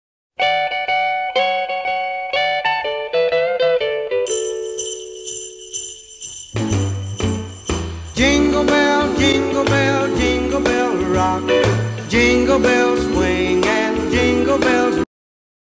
Adorable Christmas Toys! Snoopy, Mickey & More
Had so much fun on this walkthrough playing with Christmas toys in Lowe’s.
Shop with me for toys including Snoopy, Mickey Mouse & more